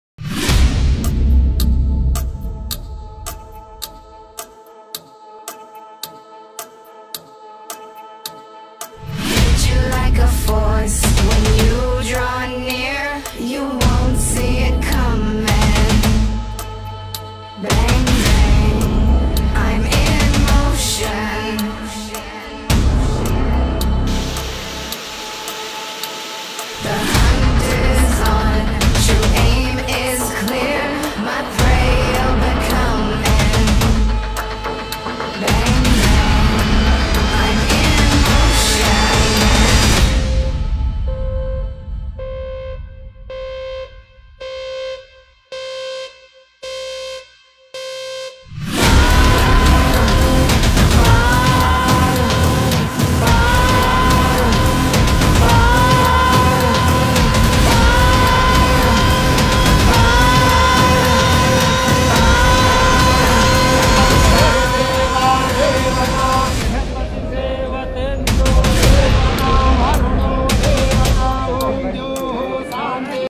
Kedarnath Temple journey by sehrill
mesmerizing view and beautiful journey to bolenath baba.  It is approximately 86 kilometres from Rudraprayag.
Kedarnath means - the Lord of the Field.